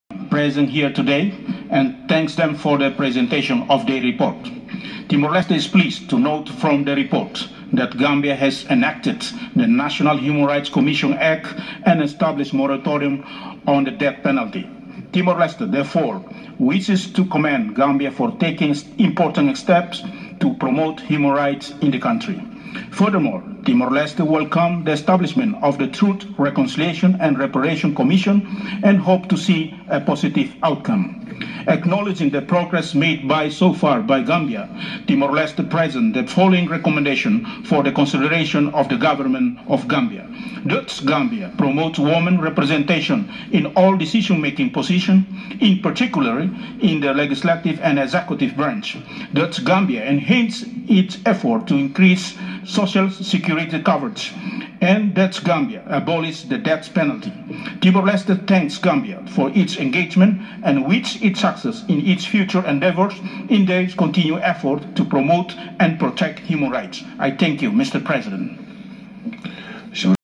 Francisco Fernandes, at the UPR Working Group. Human Rights Council
Timor-Leste statements on Gambia UPR at the 34th UPR Working Group, Human Rights Council
4-15 November 2019.